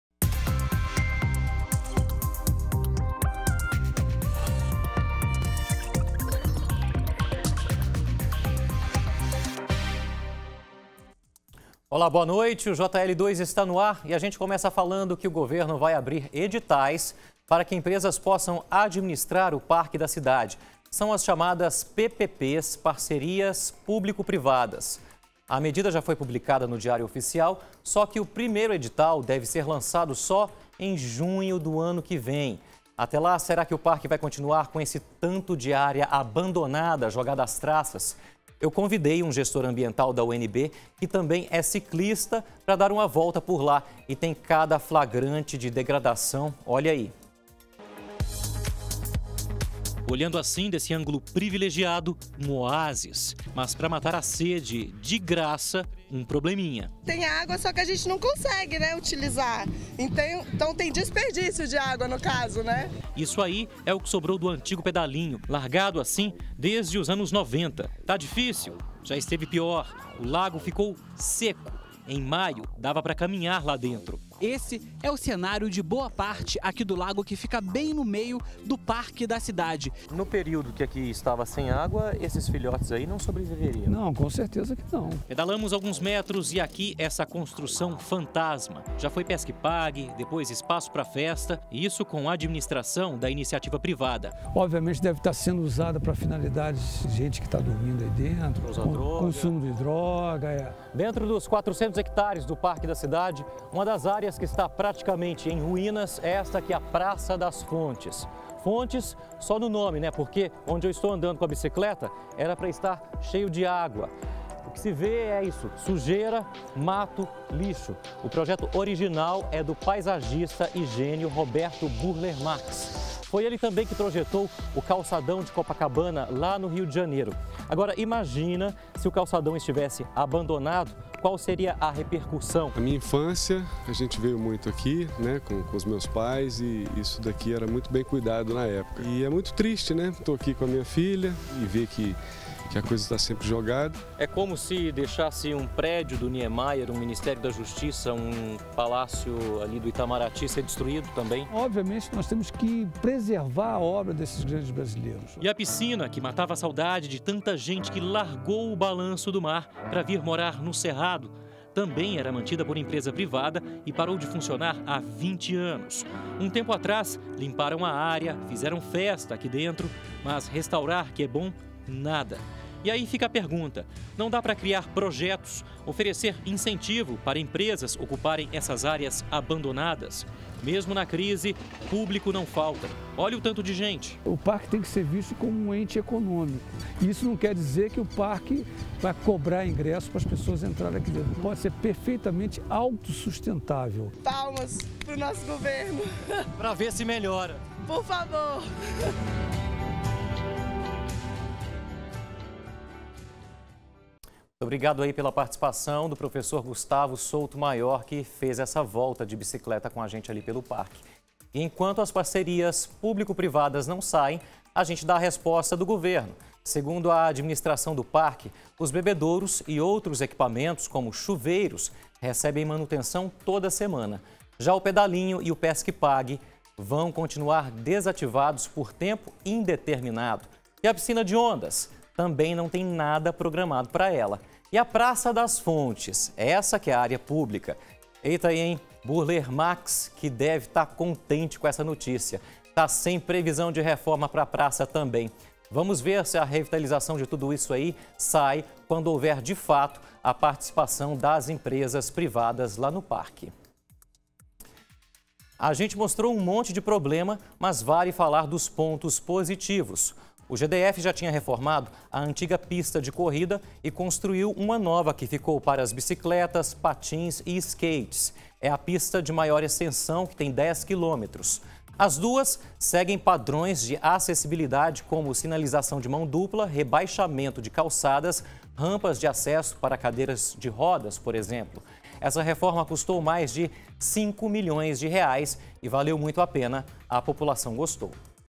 JL2 - DE OLHO NA MANUTENÇÃO
26/9 - Parque da Cidade tem áreas bem judiadas